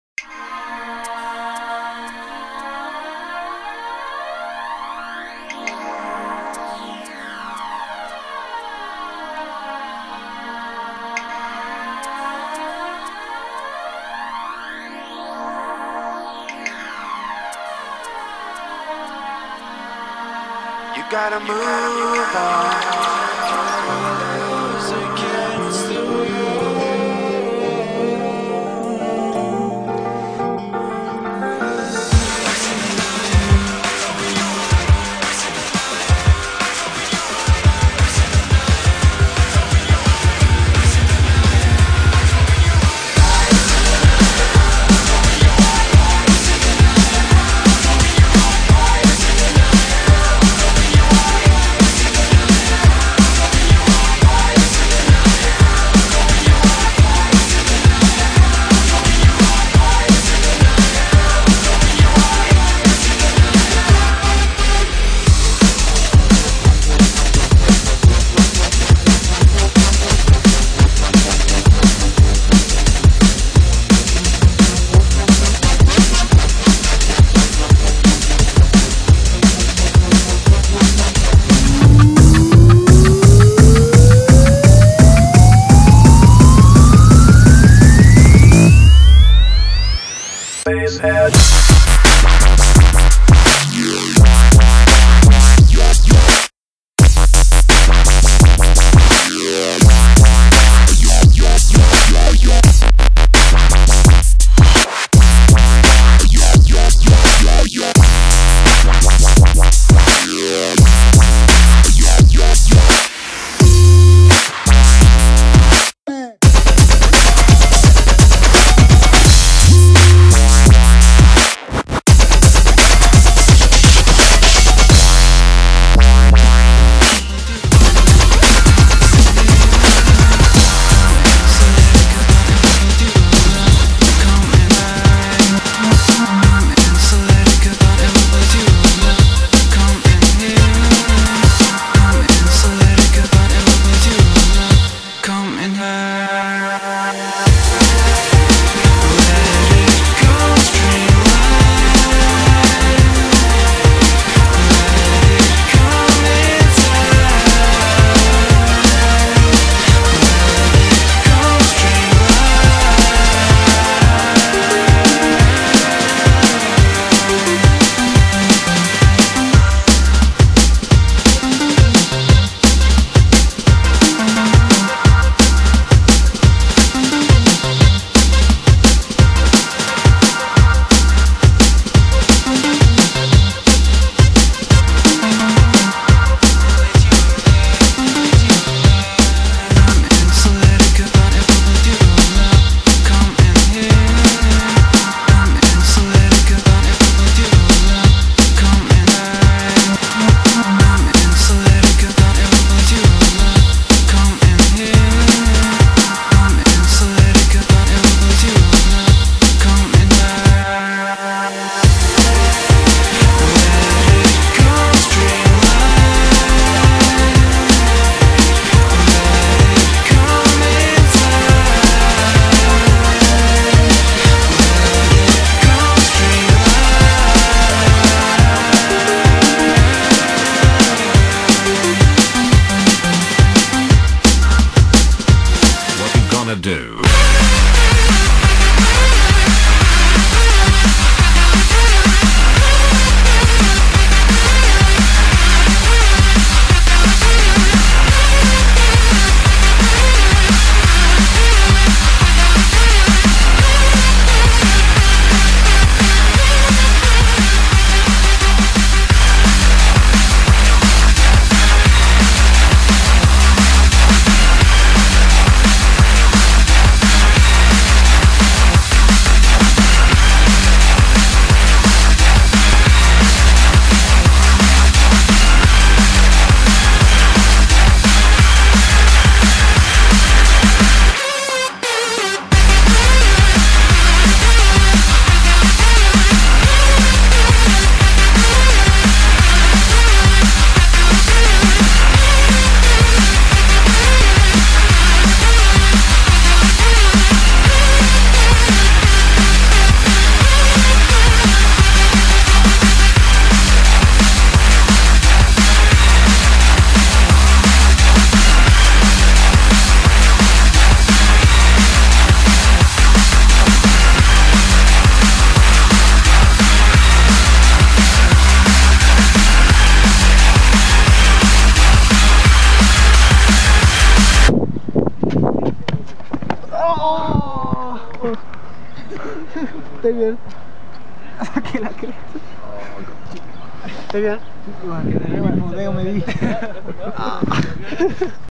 Cualquiera puede trazar!
Bueno el video nos muestra que motivado y amor a lo que se hace, cualquiera puede trazar!
Zero limite
parkour & freerunning 2010 rancagua, chile
Ramosleo69